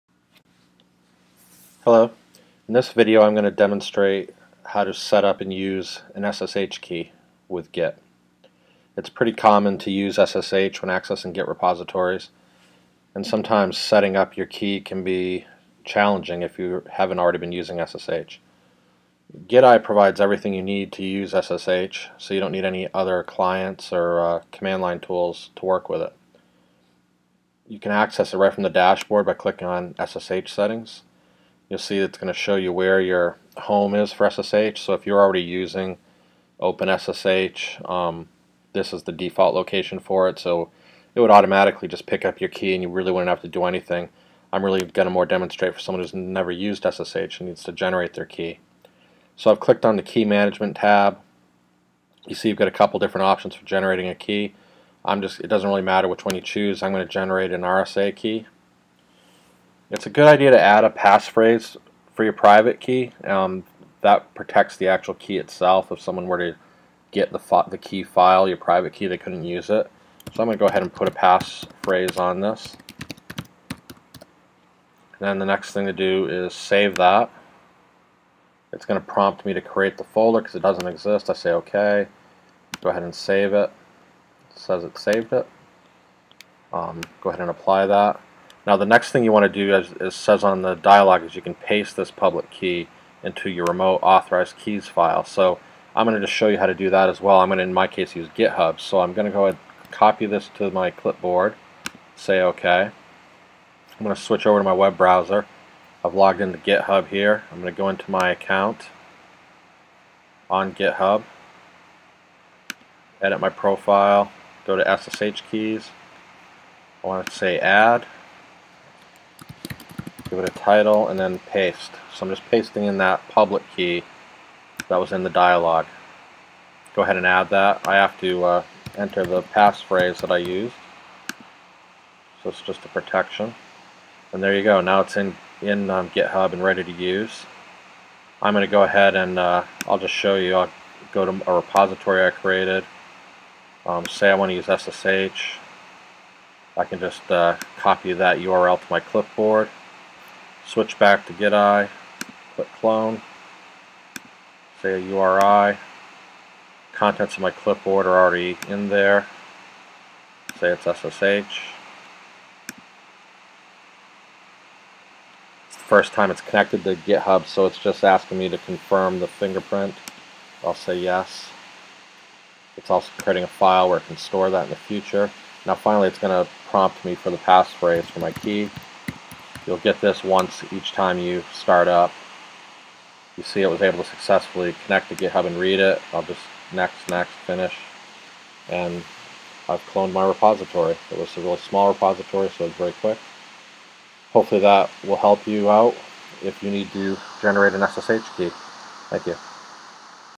GitEye SSH Key Management
Demonstrates how to create and use an SSH key using GitEye